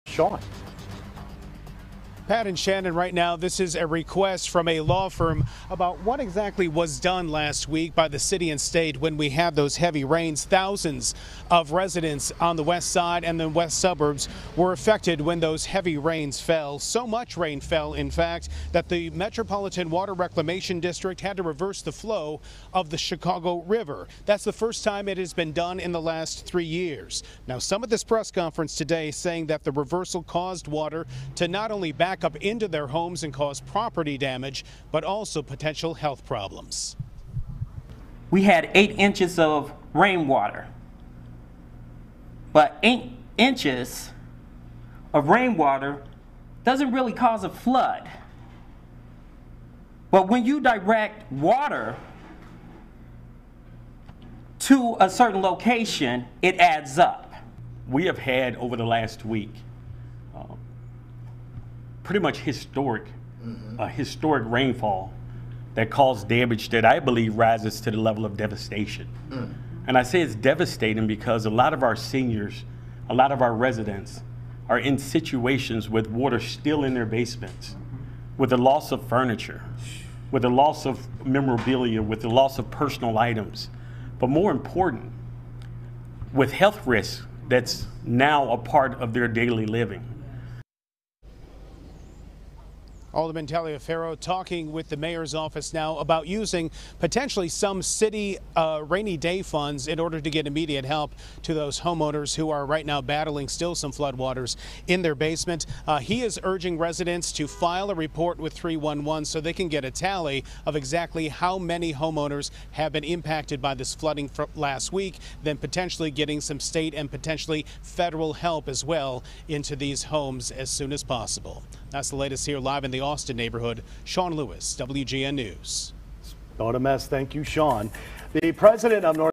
Chicago residents speak out about damage after massive floodings
A law firm is requesting information on what has been done by the city when thousands of residents on the West Side and west suburbs due to the rain last week.